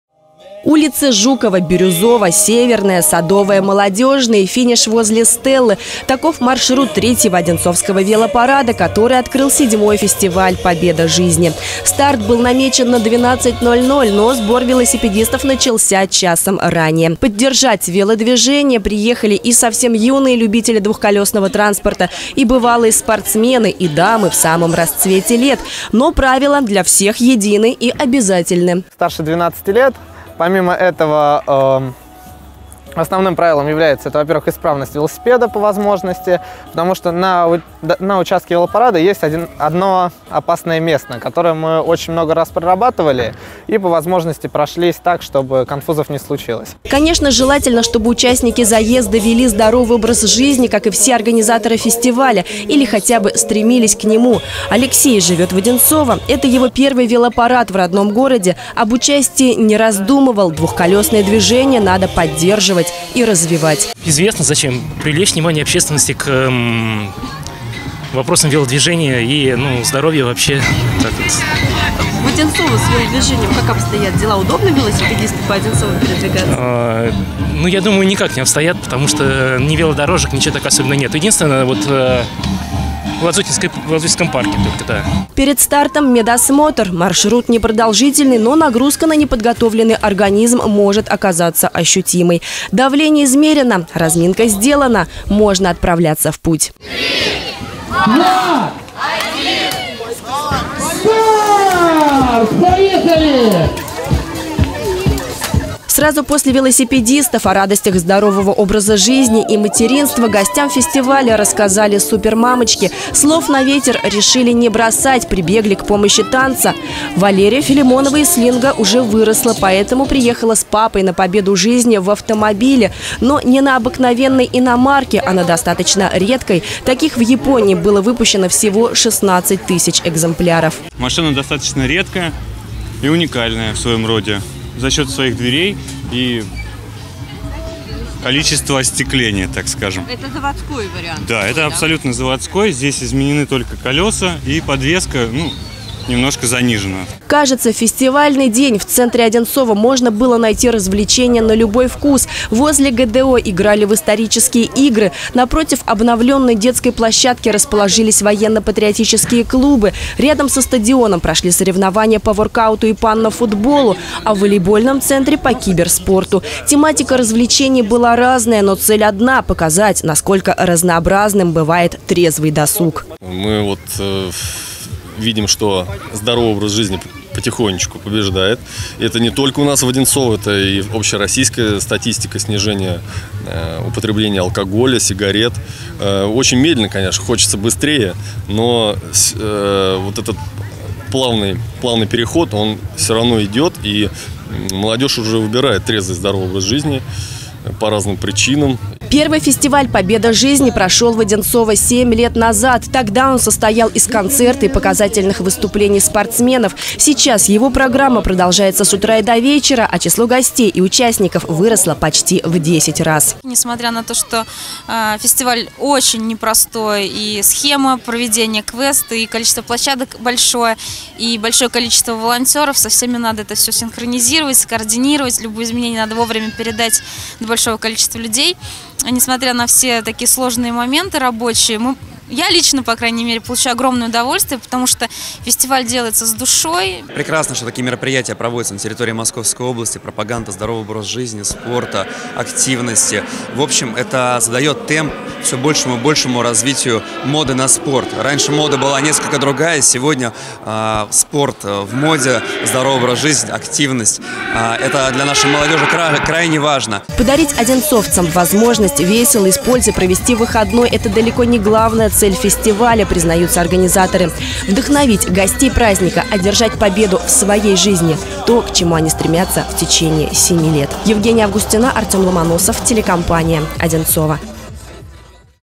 В Одинцово состоялся VII фестиваль «Победа жизни»
Лазертаг, скалодром, бампербол – выбор развлечений, как всегда, богатый. С каждым готом количество испытаний  растет, не меняется только главная цель фестиваля – показать, насколько разнообразным может быть трезвый досуг.